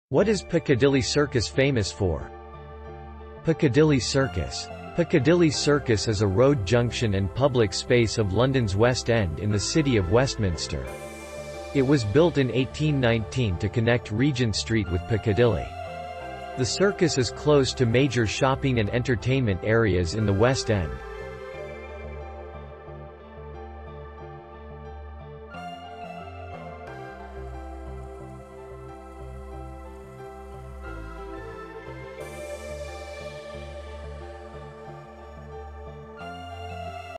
What is Piccadilly Circus famous for?
Piccadilly Circus. Piccadilly Circus is a road junction and public space of London's West End in the City of Westminster. It was built in 1819 to connect Regent Street with Piccadilly. ... The Circus is close to major shopping and entertainment areas in the West End.

Reference:
Piccadilly Circus - Wikipedia